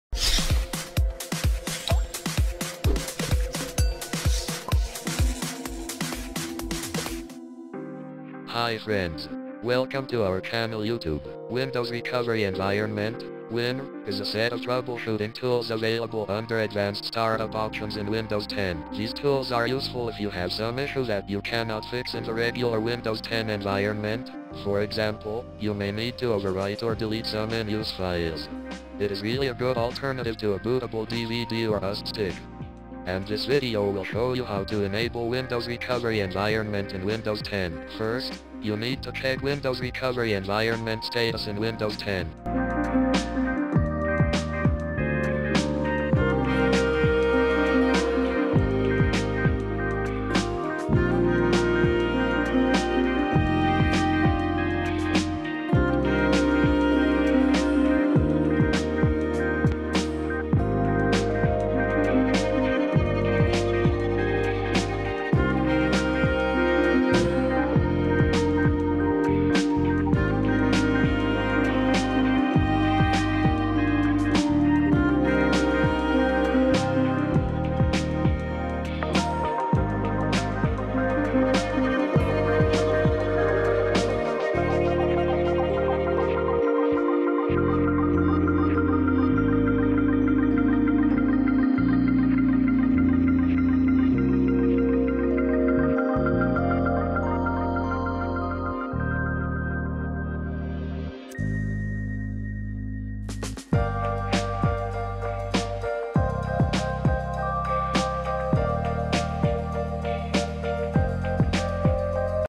How to Enable Windows Recovery Environment in Windows 10
This video will show you how to enable Windows Recovery Environment in Windows 10.
Windows Recovery Environment includes the following tools:
- Automatic Startup Recovery. If the system detects a boot failure on a computer running Windows, the system automatically fails over into the on-disk Windows RE tool.
- Automatic Repair. The Automatic Repair tool automates common diagnostic and repair tasks for non-bootable operating system installations.
- System Image Recovery.
- System Restore.
- Command Prompt.
- The ability to customize startup settings.
- It may also include custom Support and Recovery Tools from OEM.

Open Command Prompt as Administrator
To Check Windows Recovery Environment Status in Windows 10
reagentc /info
See the value next to the Windows RE Status line. It should say either Disabled or Enabled.
reagentc /enable